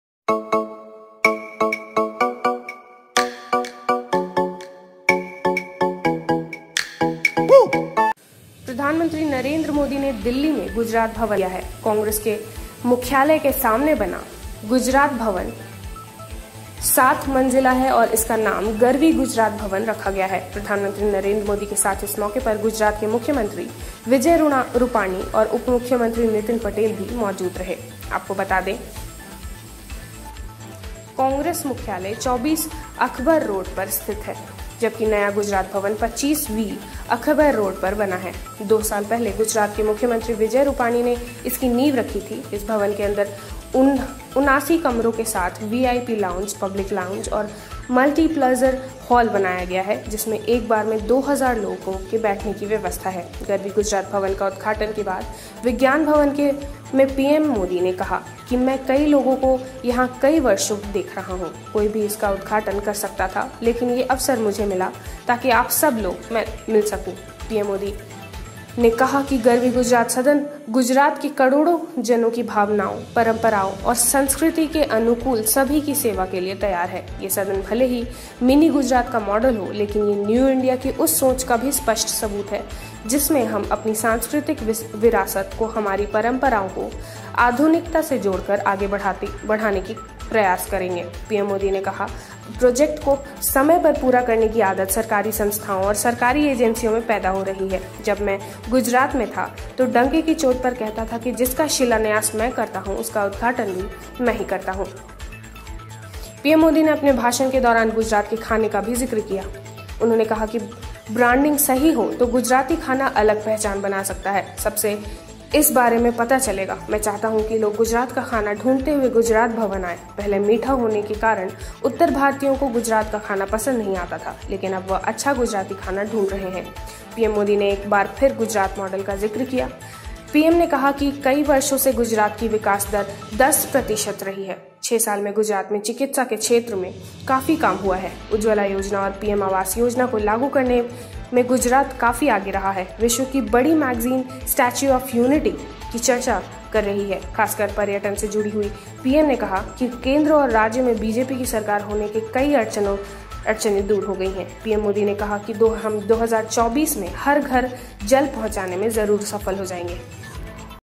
नरेंद्र मोदी ने किया दिल्ली में गुजरात भवन का उद्घाटन
Delhi me PM modi NE kiya Gujarat bhawan ka udghatan